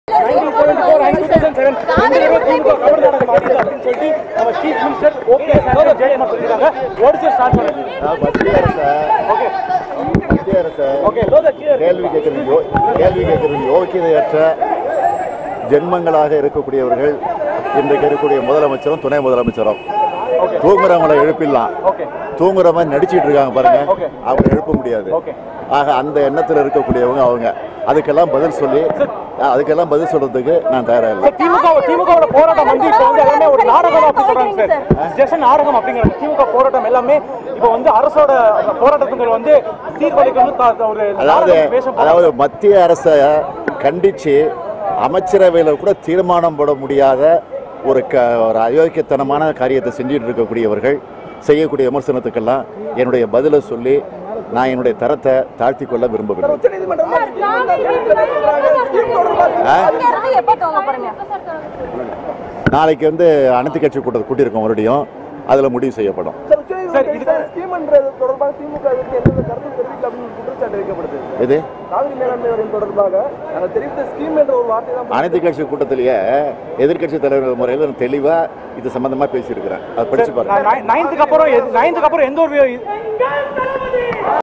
Edappadi Palanisamy is sleeping Stalin reacts tamil news live, tamil live news tamil news redpix
‘Edappadi Palanisamy is sleeping, he cannot be woken up ’, Stalin reacts tamil news live, tamil live news,  tamil news redpix

DMK working president M K Stalin reacting to Tamil Nadu’s chief minister’s comment that DMK was playing false drama on cauvery issue Stalin said’ those people sleeping can be woken up, but those acting to be sleeping cannot be woken up’.

Stalin further said, the tamil nadu government cannot even pass a resolution in its cabinet meeting condemning the central government on cauvery issue, and I don’t want to put myself down by replying to these silly allegations.